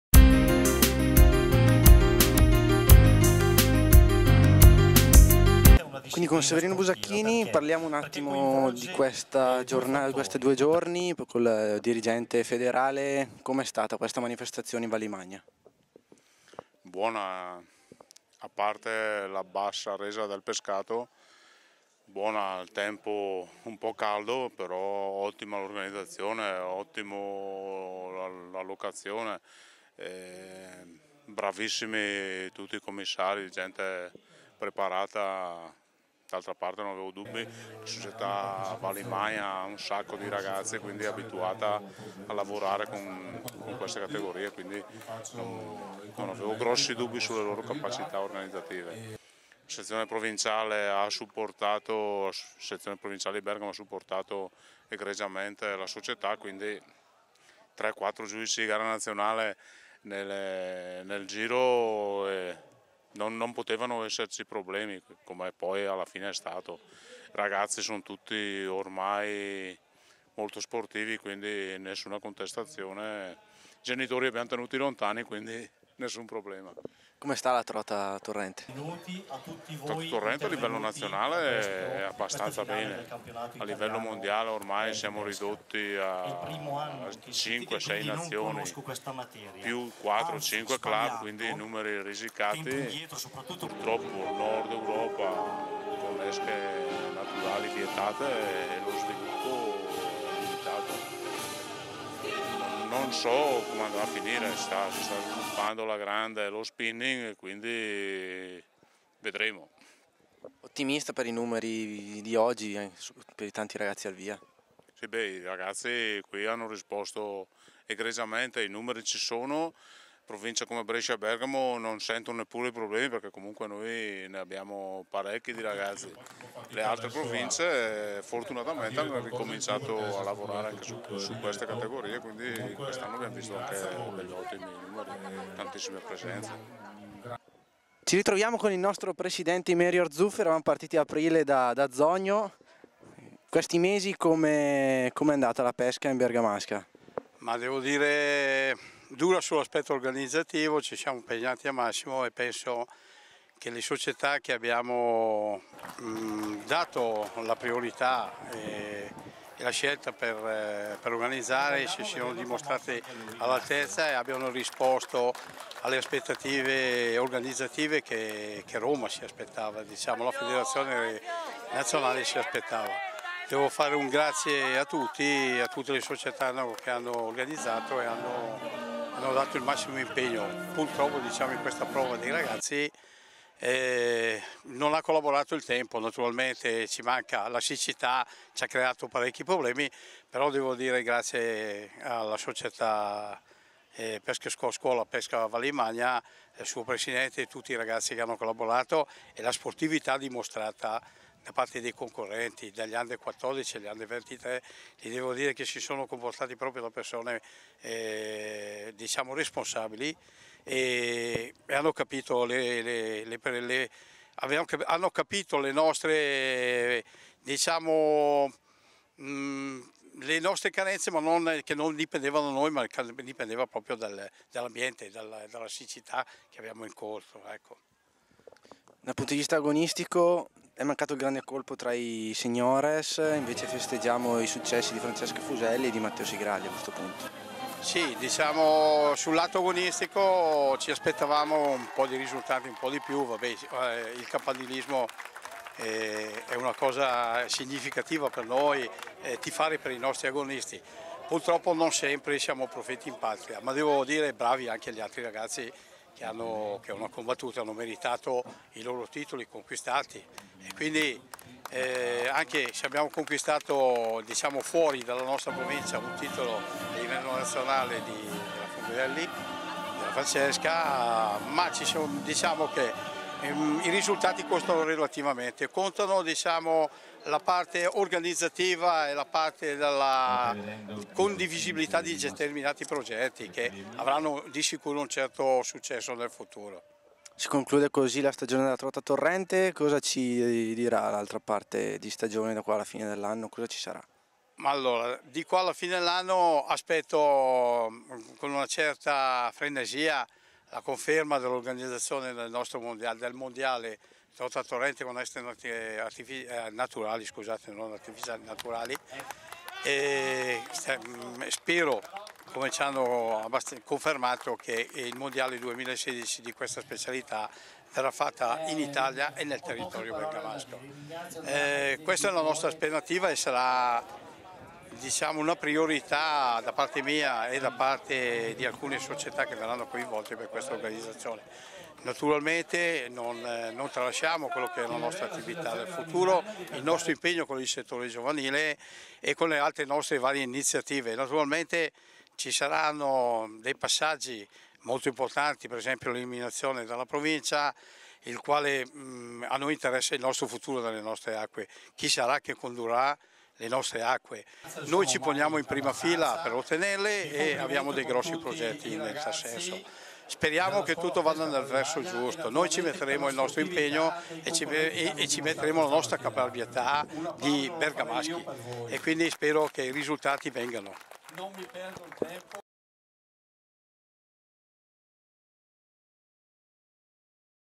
I dirigenti presenti al campionato italiano trota torrente giovanile
Intervistati il dirigente nazionale Fipsas Severino Bussacchini e il presidente provinciale Fipsas Imerio Arzuffi